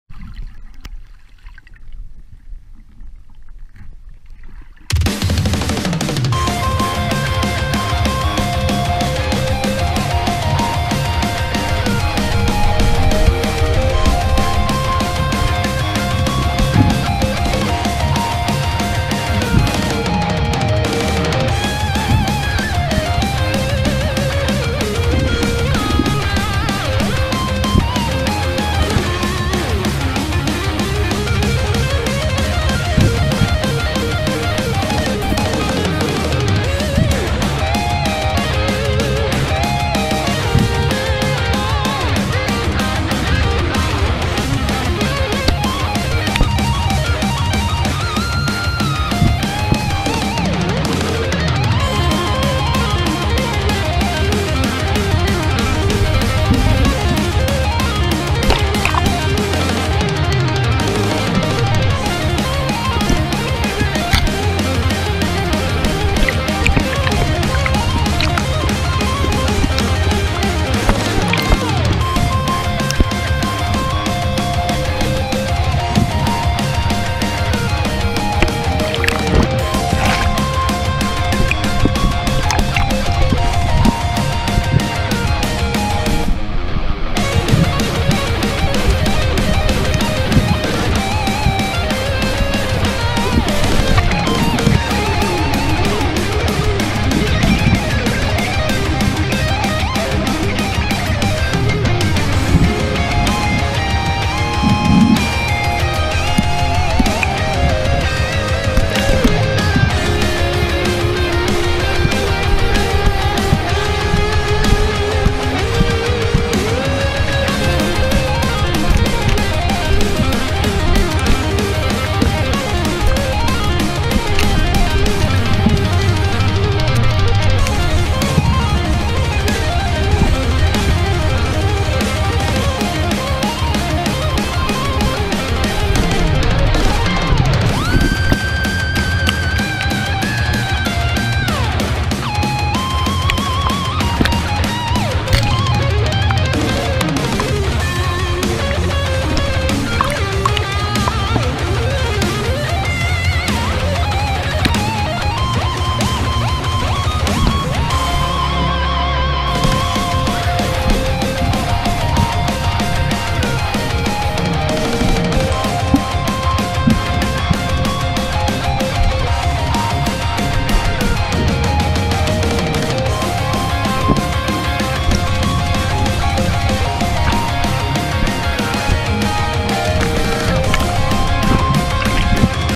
lake cormorant carp
some really big small mouth bass in a spawning bed, and spearing some pretty large carp.